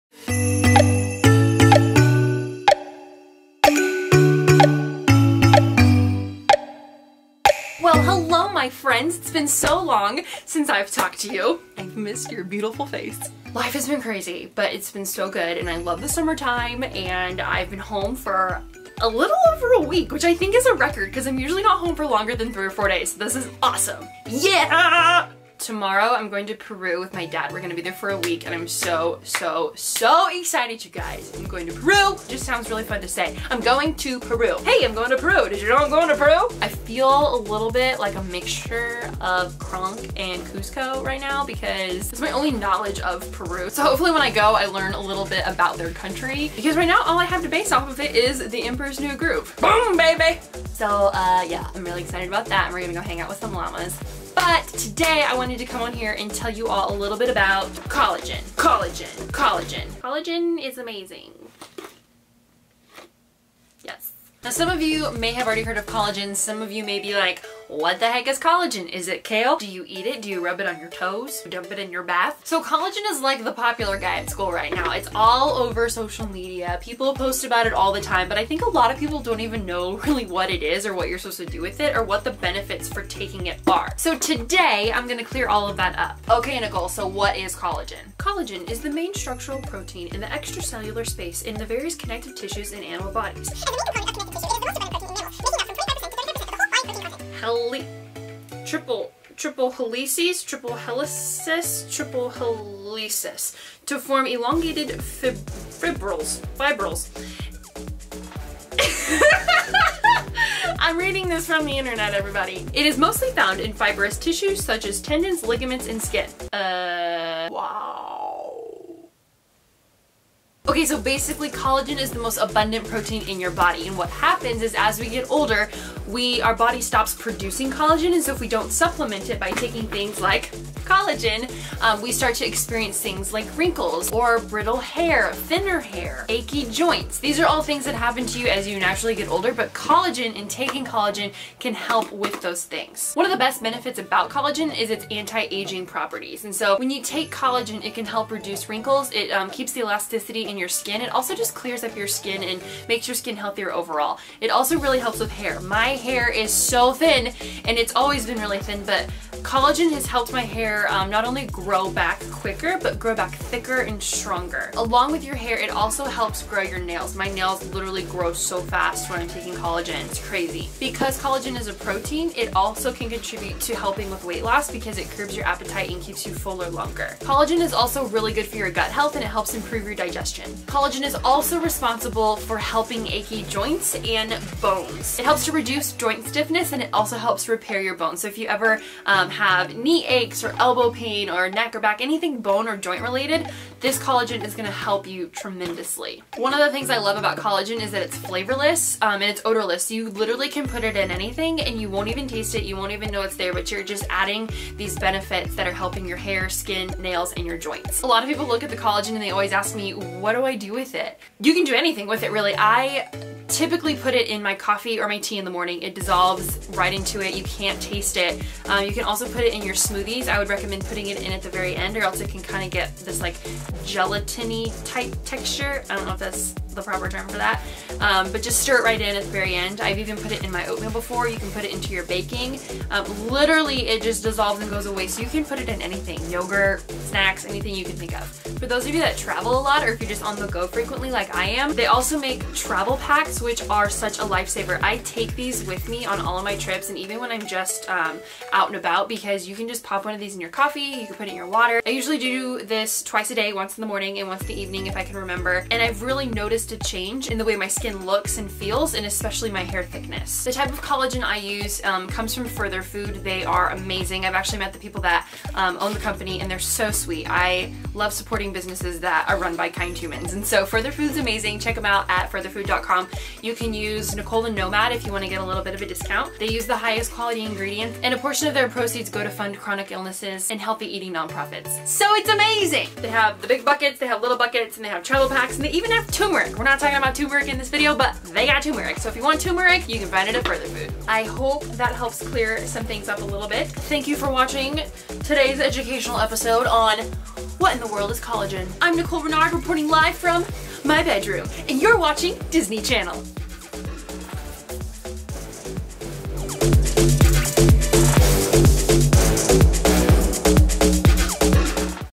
The benefits of collagen and why you should be taking it
HEY! So I'm sure you've been seeing collagen floating around the interweb & you might be wondering, what the heck is it? Basically collagen is the fairy godmother for all the problems you experience as you age; wrinkles, brittle, thin hair, weak nails, achy joints etc.

Just a serving of this a day can dramatically impact your wellbeing and make you feel like you're 25 when you're 70 (I haven't experienced that, but I've heard rumors;)

If you'd like to try some, check out this link and use "NICOLETHENOMAD" to get a discount! Share it with all your friends so you and your pals can be wrinkle free!

See you real soon!

xx Nicole